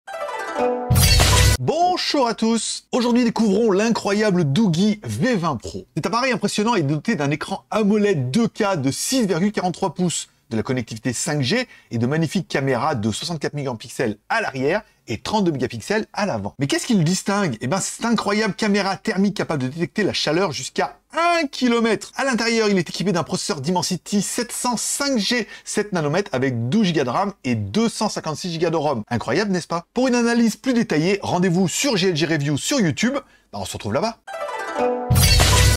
Doogee V20 Pro : Le Futur de la Technologie Mobile !
Découvrez le Doogee V20 Pro, avec son incroyable écran AMOLED 2K, sa connectivité 5G et ses caméras de 64 et 32MP. Vous serez impressionné par sa caméra thermique innovante. Rendez-vous sur GLG Review pour plus de détails !

By GLG